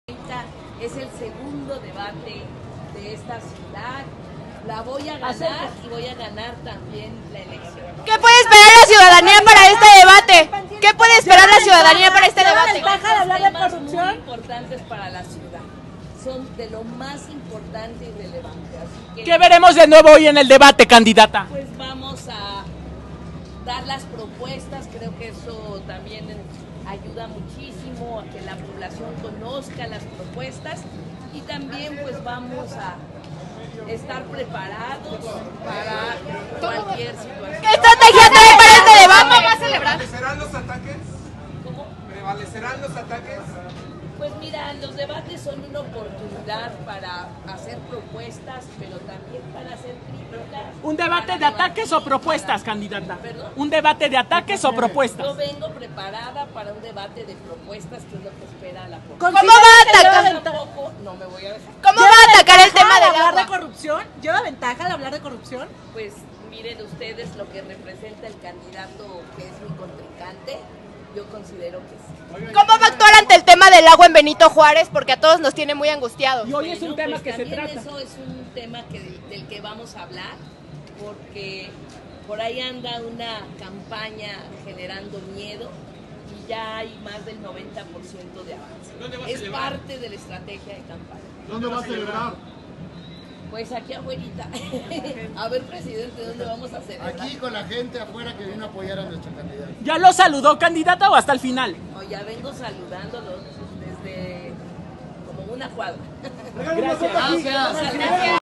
Llega la candidata Clara brugada al 2do debate chilango.
Así fue la llegada de la candidata por la coalición “sigamos haciendo historia” Clara Brugada Molina, del partido de morena a las instalaciones de mvs televisión, con motivo del 2do debate chilango.